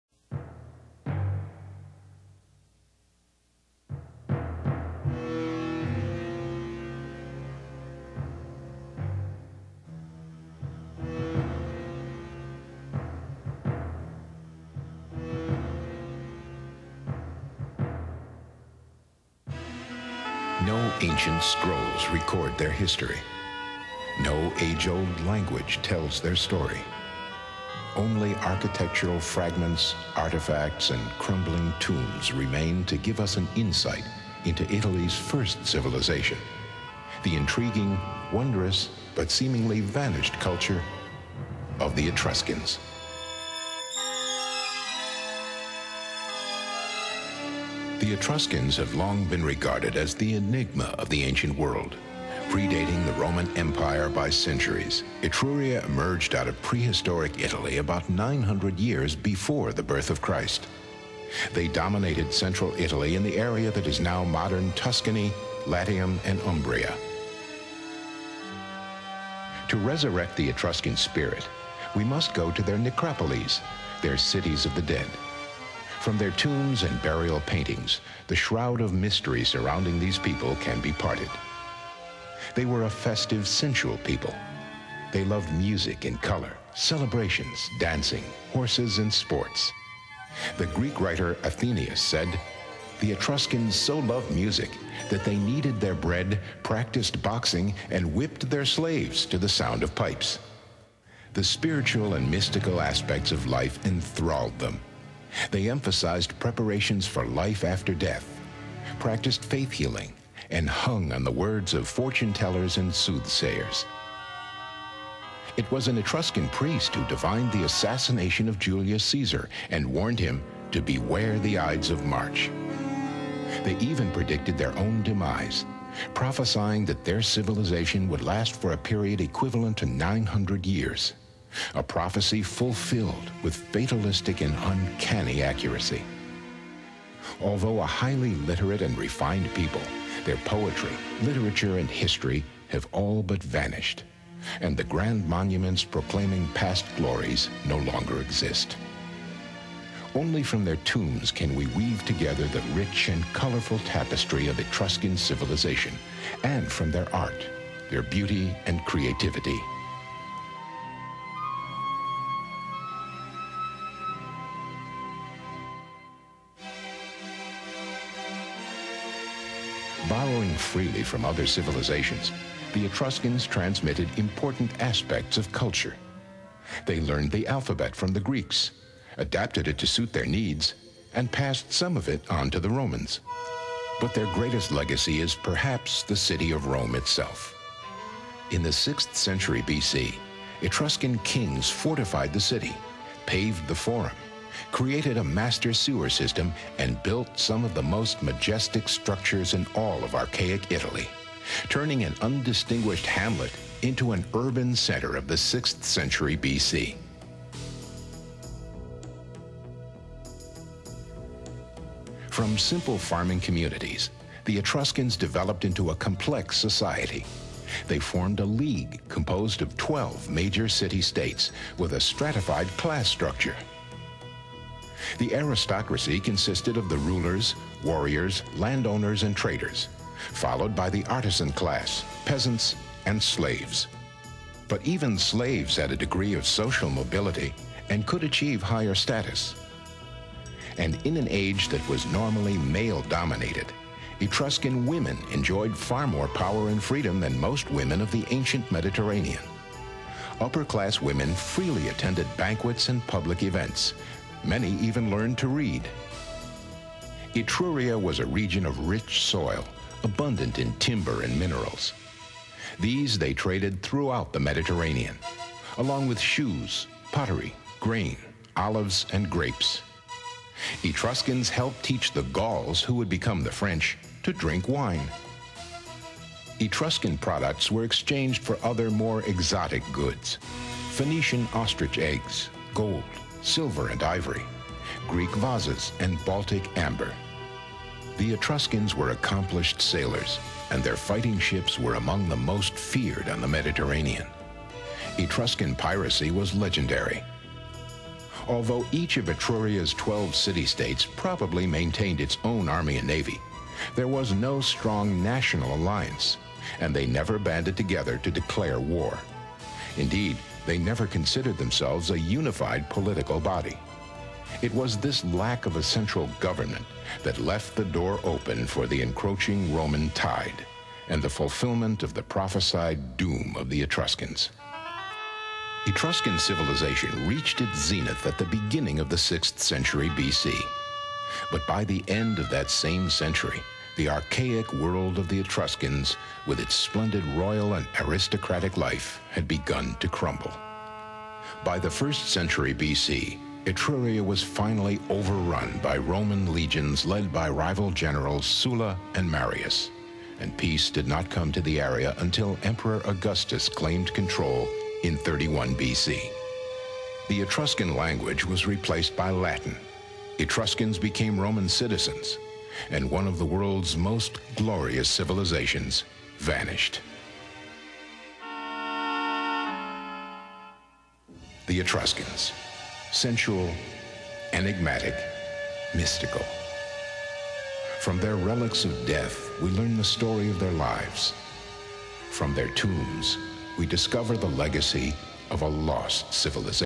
The Etruscans - Legacy of a Lost Civilization
This orientation video was part of the Memphis Wonders Series at Pink Palace Museum in Memphis, TN.  Footage was filmed and edited by Calvin Dean, and produced by Michael Baldridge for Baldridge Studios in Memphis. Shot in various "Cities of the Dead" including Cerveteri, Norcha, Orveito and Rome, Italy, this video explores the enigmatic people who predated the Romans.